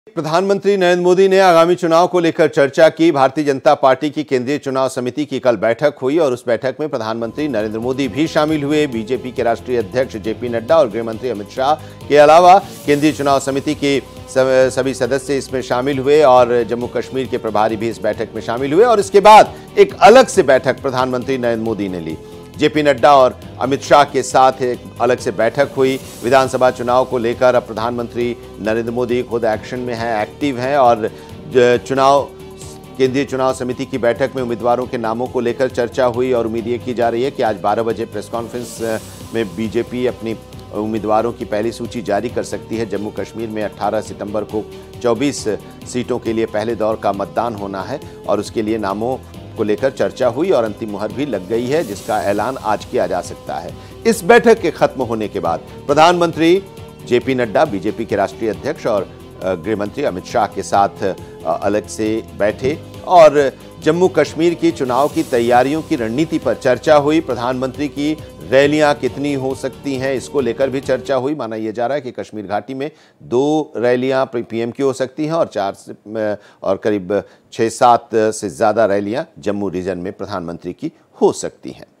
PM Modi ने BJP CEC की बैठक के बाद एक अलग से बैठक ली, आगामी चुनाव पर हुई चर्चा | Breaking News
Ronak Rathi